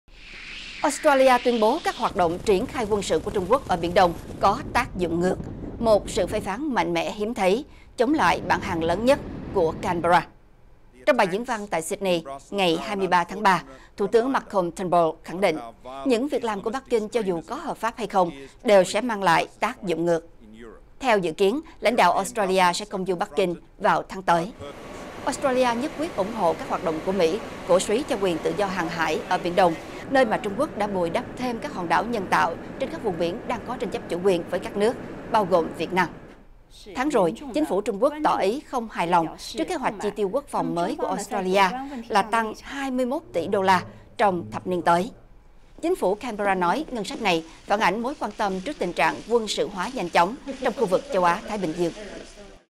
Australia: Hành động của TQ ở Biển Đông ‘tác dụng ngược’
Tin tức Nếu không vào được VOA, xin các bạn hãy vào để vượt tường lửa. Thủ tướng Australia tuyên bố các hoạt động triển khai quân sự của Trung Quốc ở Biển Đông có tác dụng ngược, một sự phê phán mạnh mẽ hiếm thấy chống lại bạn hàng lớn nhất của Canberra.

Trong bài diễn văn tại Sydney hôm nay Thủ tướng Malcolm Turnbull khẳng định những việc làm của Bắc Kinh cho dù có hợp pháp hay không đều sẽ mang lại phản ứng ngược.

Theo dự kiến, lãnh đạo Australia sẽ công du Bắc Kinh vào tháng tới.

Australia nhất quyết ủng hộ các hoạt động của Mỹ cổ súy quyền tự do hàng hải ở Biển Đông, nơi Trung Quốc đã bồi đắp thêm các hòn đảo nhân tạo trên các vùng biển đang có tranh chấp chủ quyền với các nước bao gồm Việt Nam.

Tháng rồi, chính phủ Trung Quốc tỏ ý không hài lòng trước kế hoạch chi tiêu quốc phòng mới của Australia tăng 21 tỉ đô la trong thập niên tới.

Chính phủ Canberra nói ngân sách này phản ánh mối quan tâm trước tình trạng quân sự hóa nhanh chóng trong khu vực Châu Á-Thái Bình Dương.